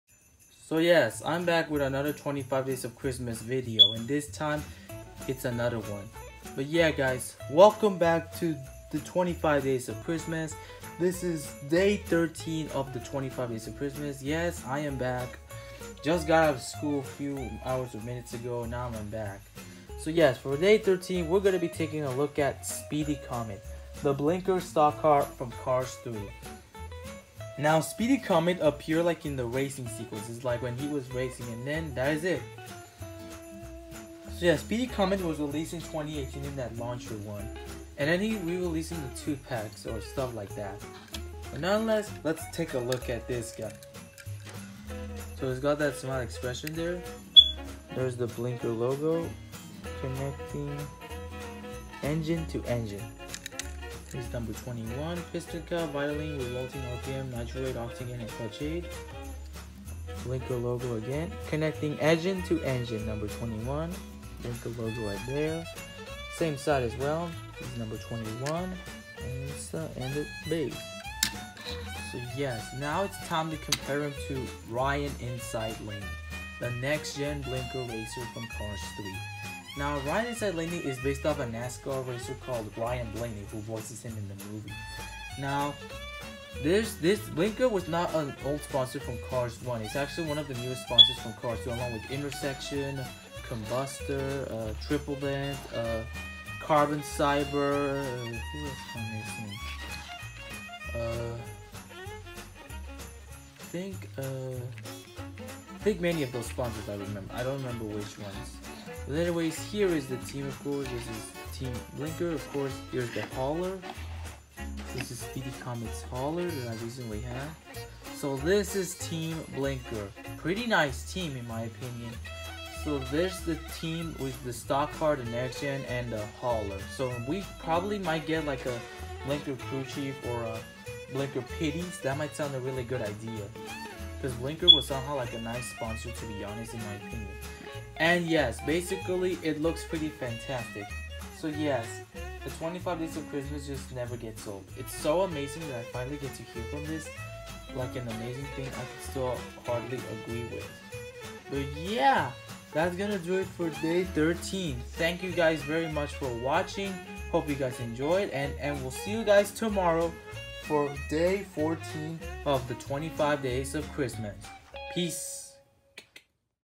25 Days Of Christmas Day 13: Speedy Comet (Blinkr #21)
Speedy Comet Was The Blinkr Stock Car At The Beginning Of Cars 3 He Doesn’t Speak And Only Appears As A Background Character. Speedy Comet Then Gets Replaced By The Next Gen Blinkr Racer Ryan Inside Laney. He Was First Released In 2018 In A Launcher Set But Also Re Released As Well.